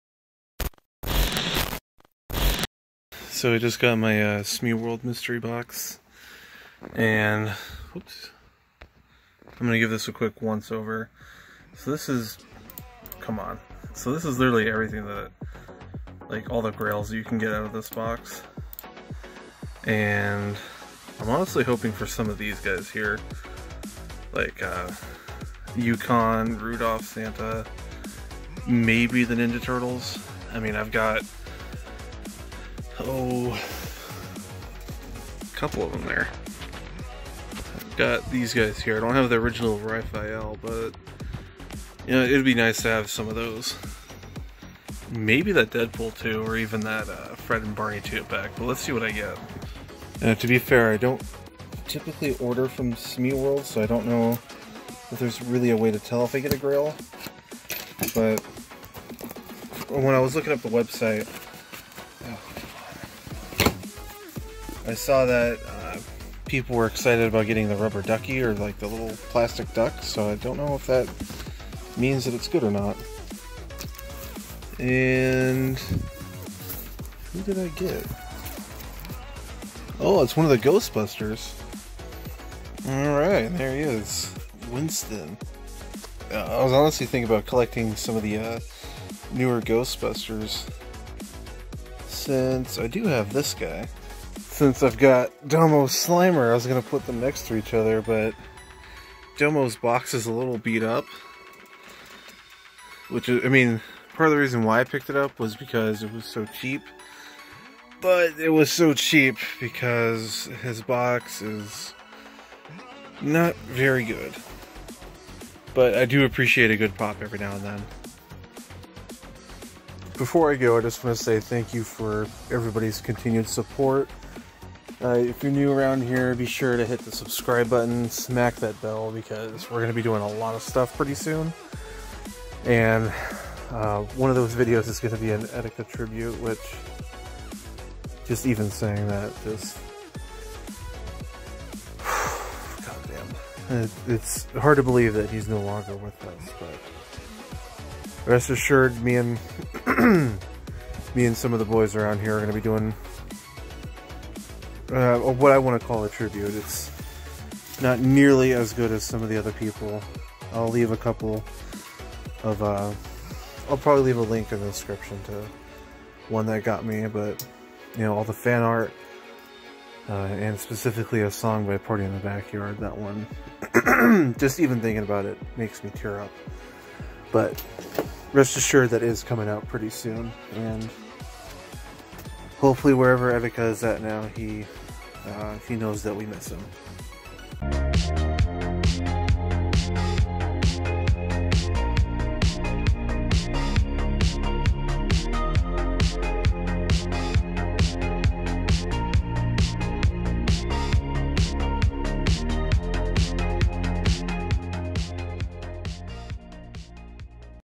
I GOT THIS... GRAIL OR FAIL? - Smeye World Mystery Box
I decided to take my chances with another box from Smeye World. Was it worth the price? Value wise, it wasn't.